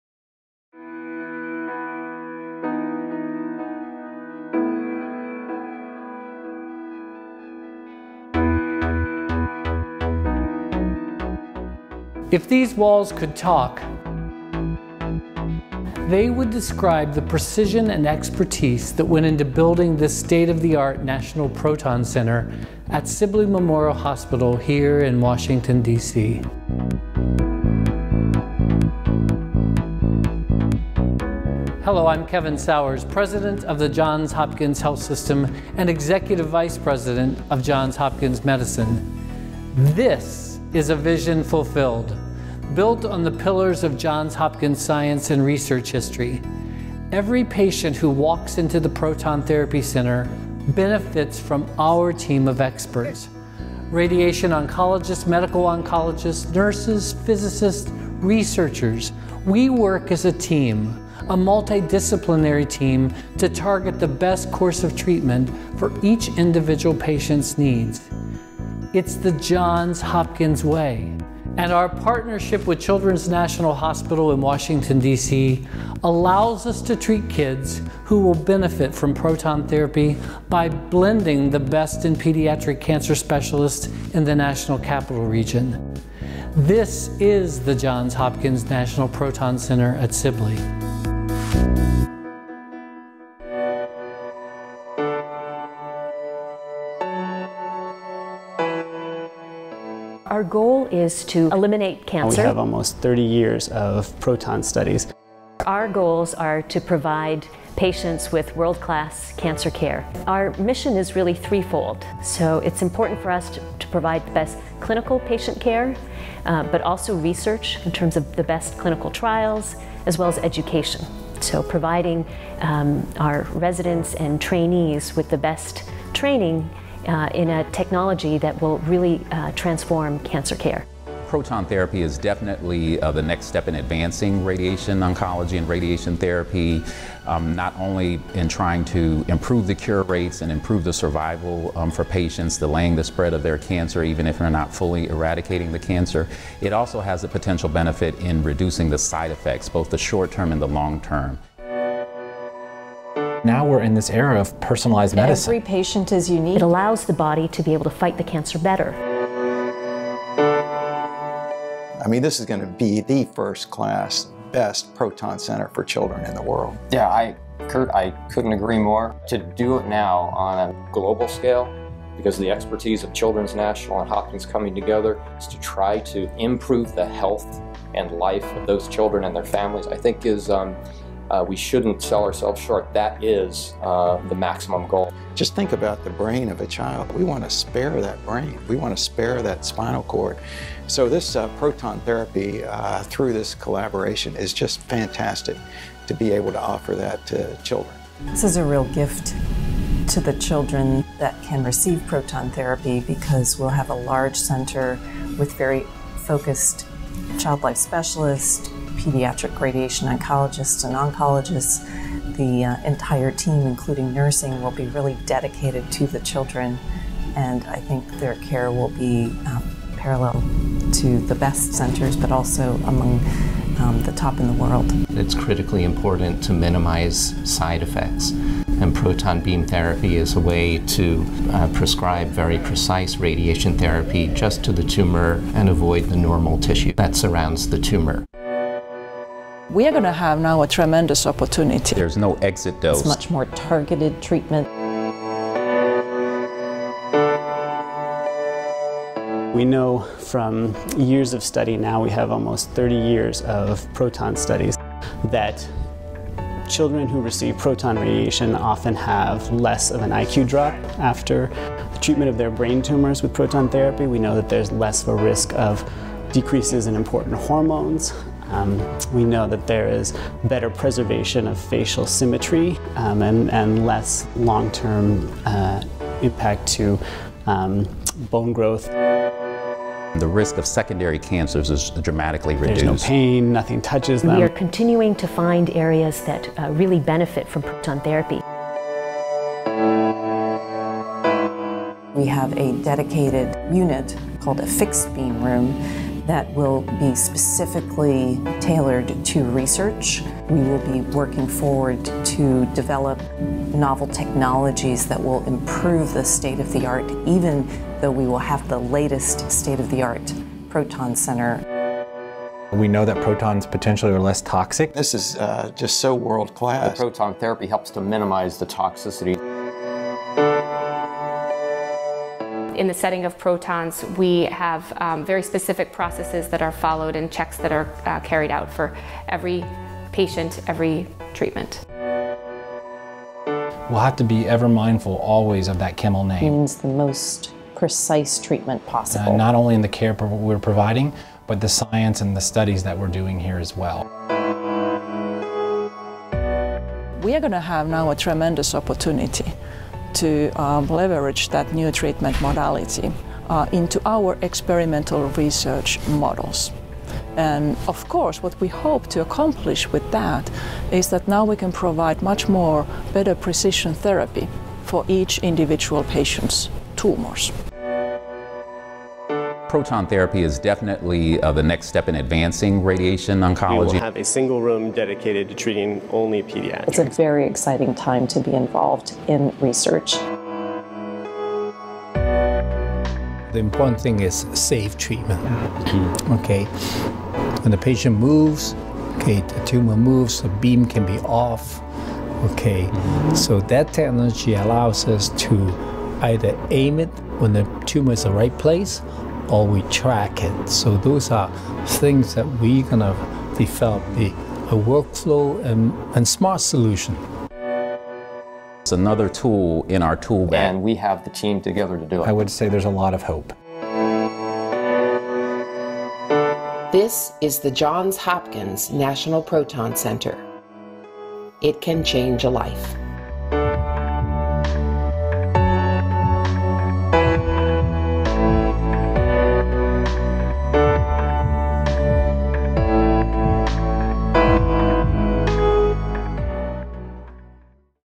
We Speak Proton
The Johns Hopkins Proton  Therapy Center at Sibley Memorial Hospital offers patients the most precise dose of radiation possible, limiting the exit dose and sparing healthy tissue. Our collaboration with Children's National Hospital makes this essential therapy option available for pediatric patients from all around the world. The proton center is also equipped with research facilities that will propel this unique technology far into the future, opening up even more opportunities to eradicate cancer and save lives.